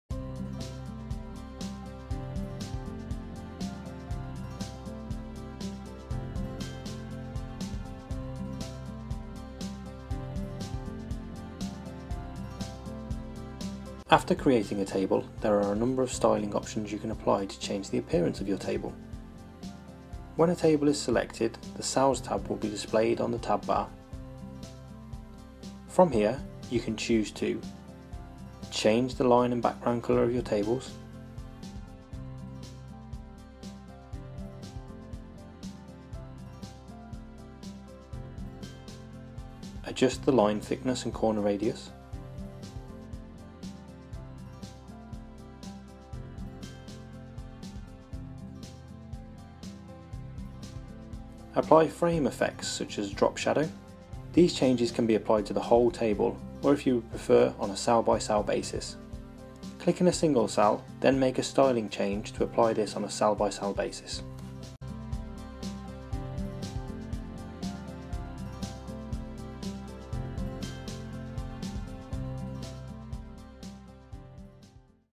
InPrint 3 Tutorial - Style a Table
After creating a table there are a number of styling options you can apply to change its appearance.